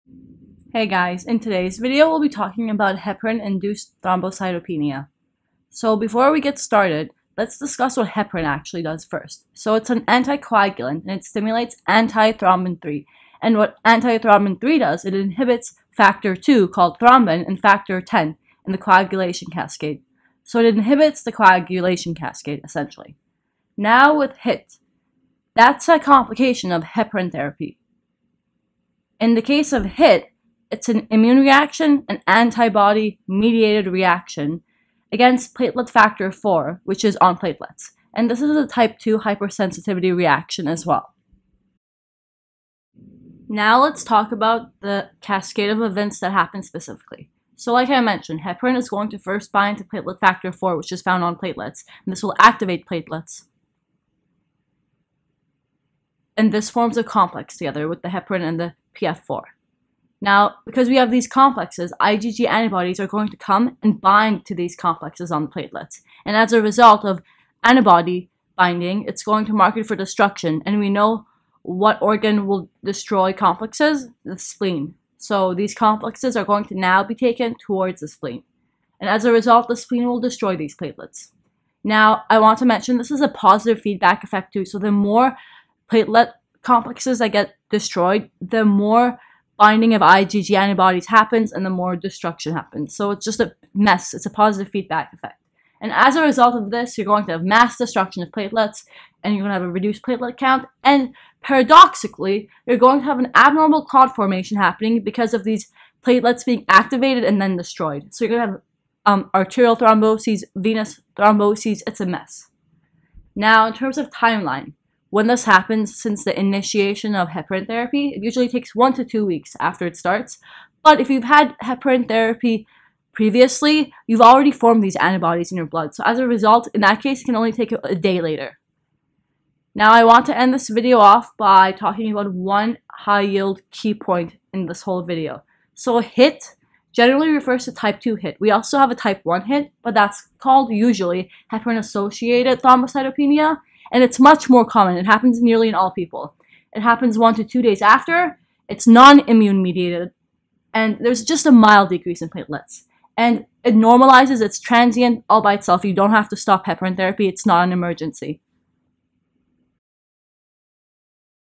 Heparin Induced Thrombocytopenia
A discussion about heparin induced thrombocytopenia and the pathophysiology of this complication of heparin therapy. HIT refers to type 2 immune mediated HIT, where as type 1 HIT is more frequently called heparin-associated thrombocytopenia and it is much more common and less severe.

NOTE:
One thing I want to make sure I clarify, although there is an overall reduced platelet count due to the mass platelet destruction, there is ALSO the excessive activation of the remaining platelets in the blood, which causes the blood to become hypercoagulable overall. As a result, despite the low platelet count, the blood will become more prone to abnormal clot formation, with both venous and arterial thromboses forming in the presence of a low platelet count. This is a paradoxical outcome.

Medical Minutes gives you high yield medical content in only a few minutes. Don’t waste time you don’t have.